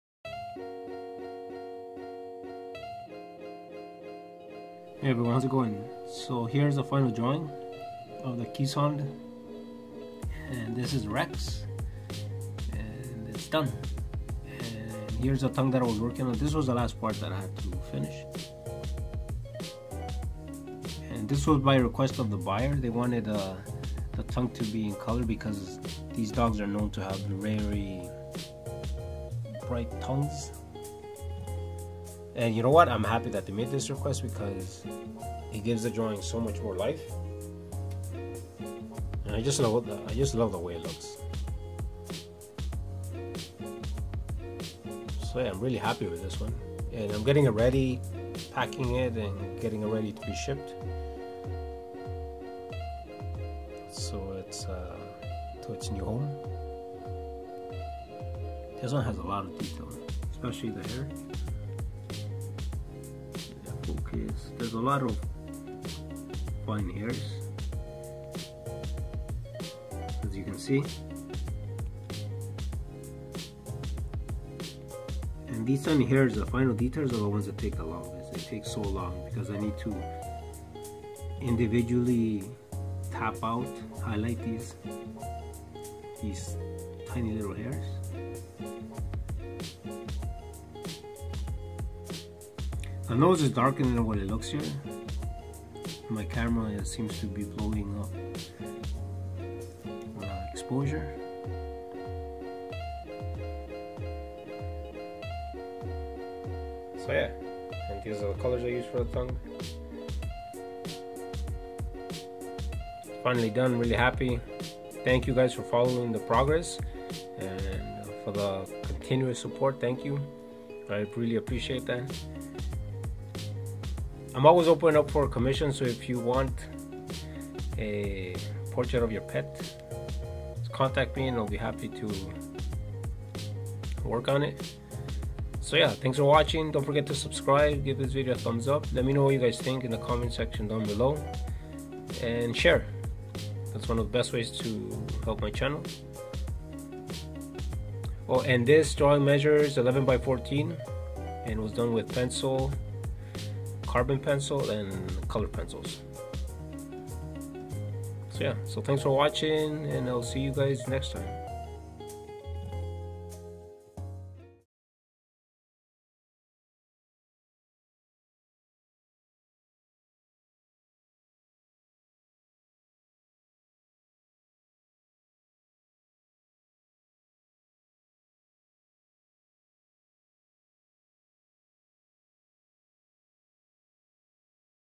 [Rex] Keeshond Pencil Drawing
This commission will have a color at the request of the client. I'm excited for this one because it will have the type of hair I love to draw.